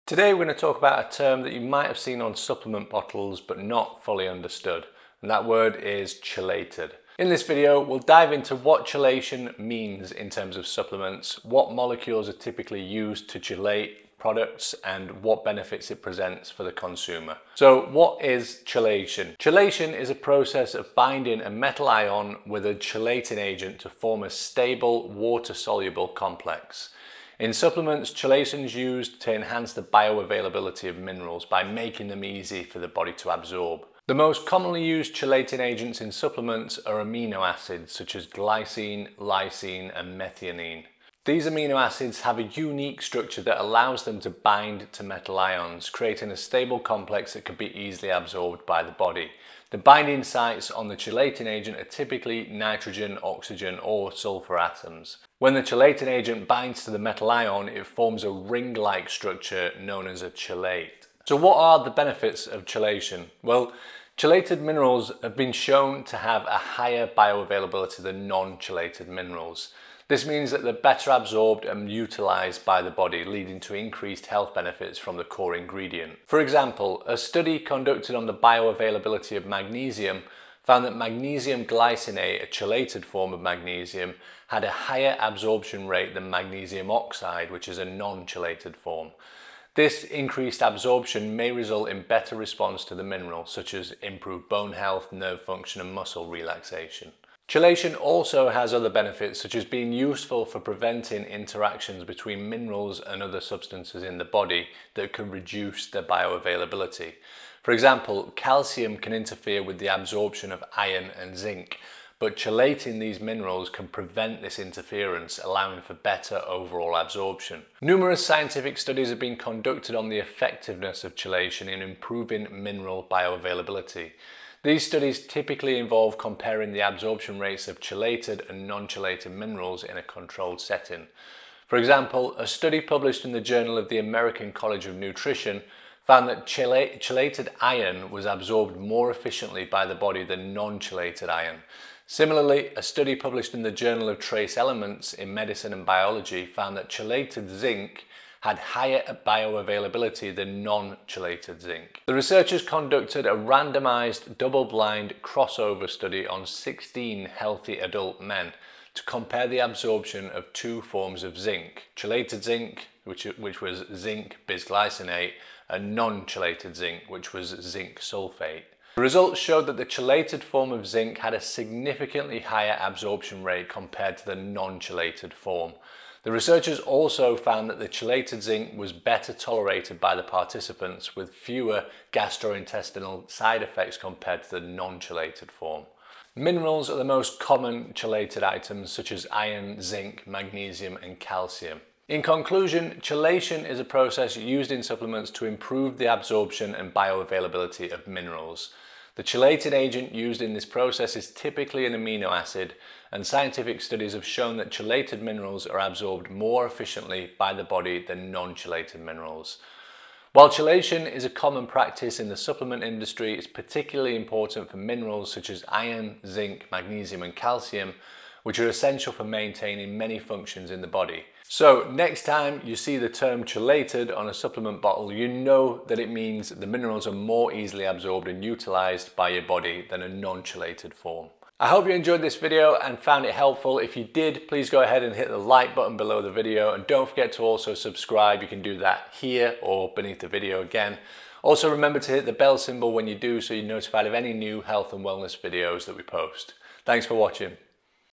WHAT DOES CHELATED MEAN on Supplements?
We dig into what the meaning of 'chelated' is in the context of supplements - covering what the process is, what's typically used to chelate minerals, and what the benefits of chelation are for the consumer.